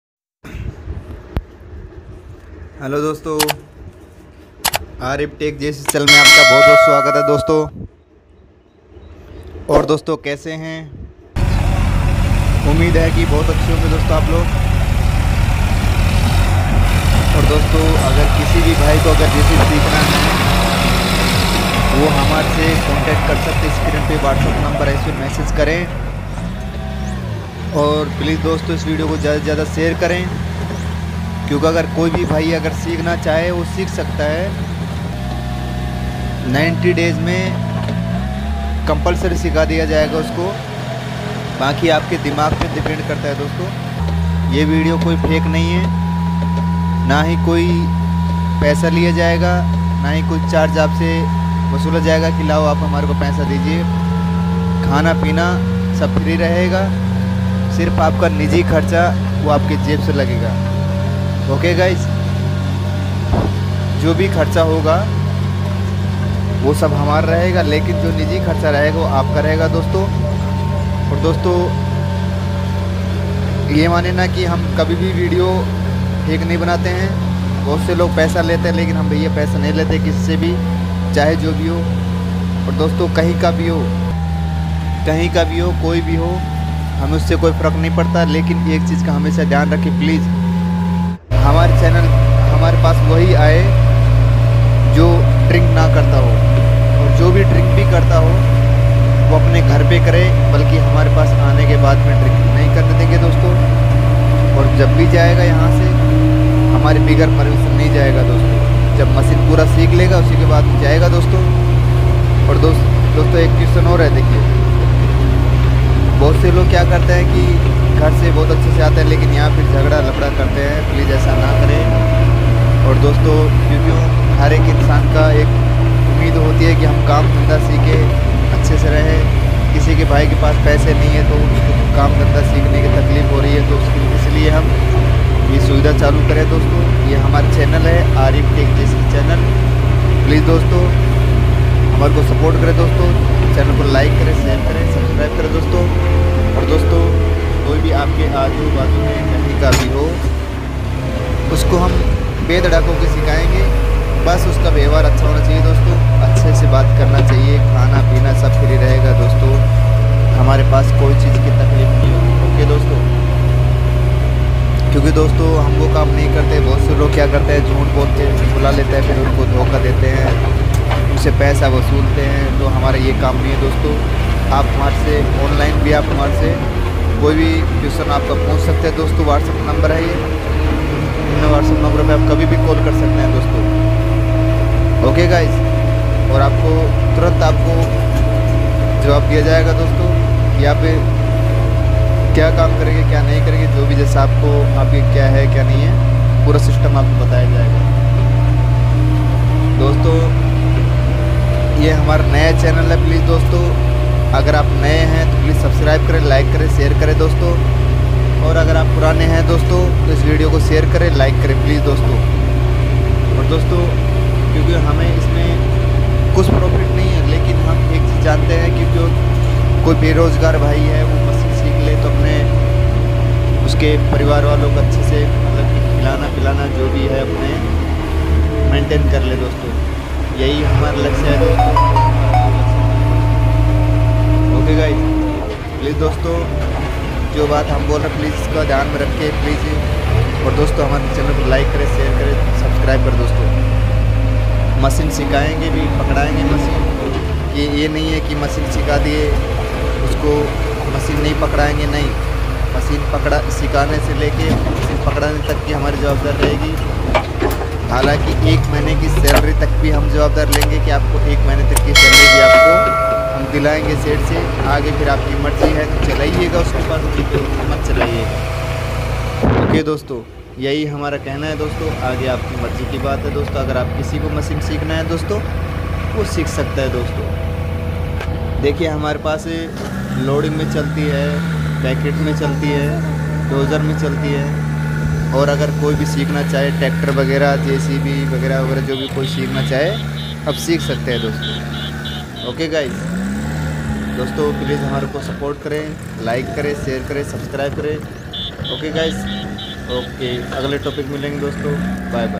3dx JCB How To Load Dumper |And Tractor With JCB Backhoe Loader JCB se kaise load kare daampar,tipar
How To Load Dumper And Tractor With JCB Backhoe Loader

jcb se kaise kareri load dumpaar
जेसीबी बैकहो लोडर के साथ डंपर और ट्रैक्टर कैसे लोड करें